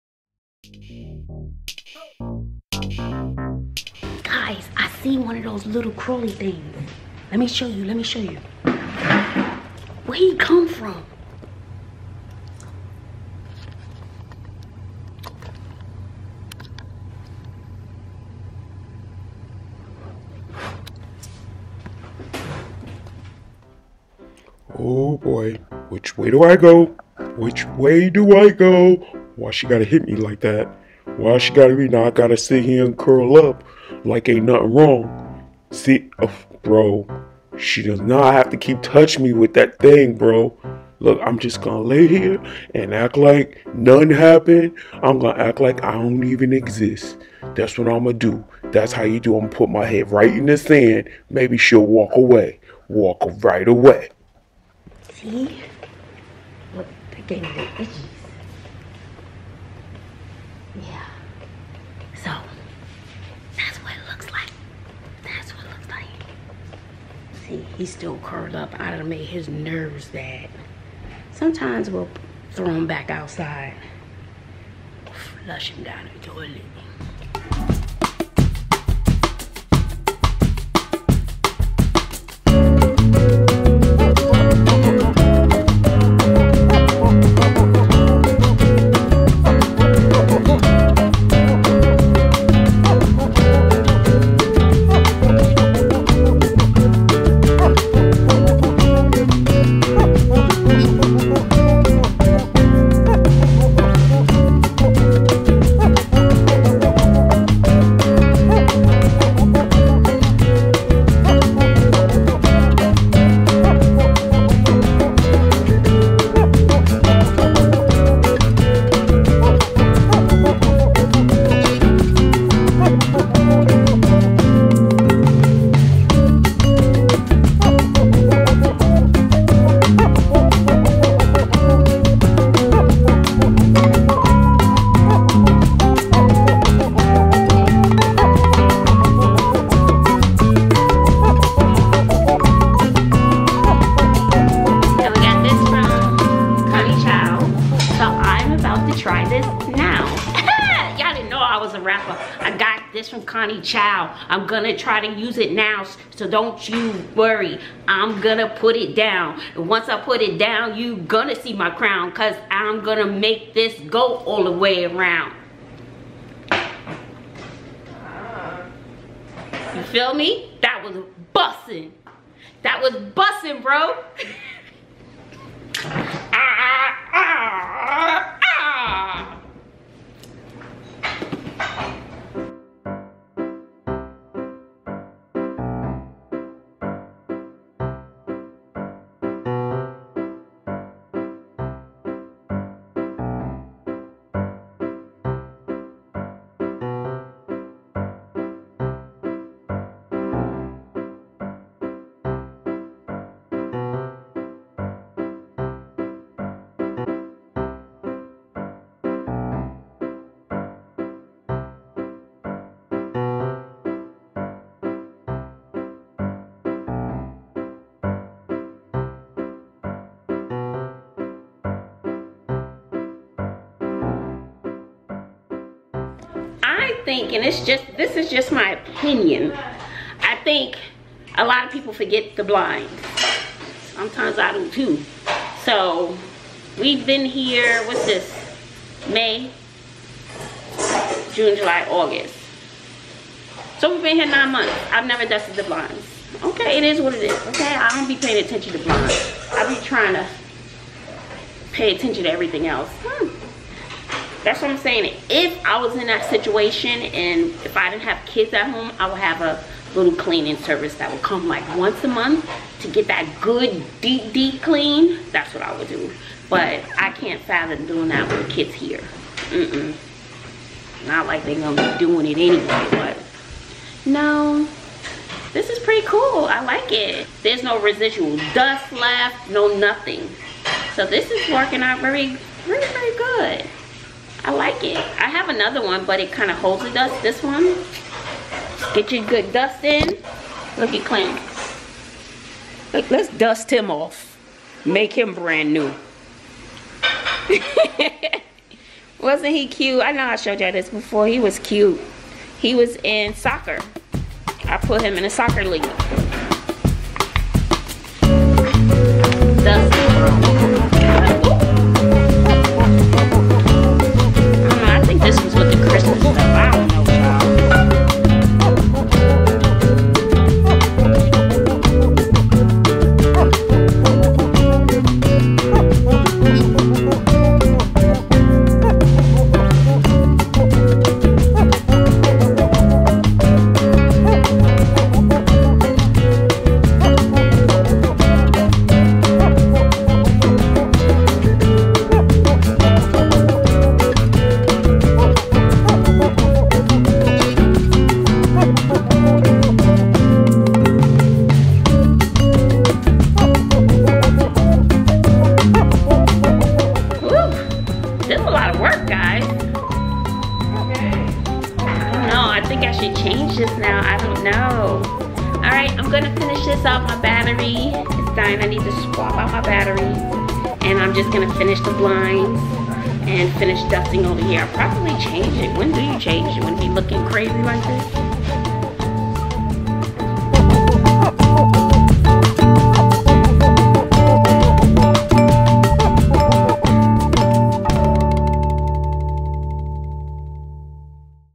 THESE BUGS HAVE TAKEN OVER MY HOME | DISGUSTING | CLEAN WITH ME
What's up Bazzle Babes!

I finally got this little critter on camera...how gross.  I have no idea where they come from, but it's about to go down.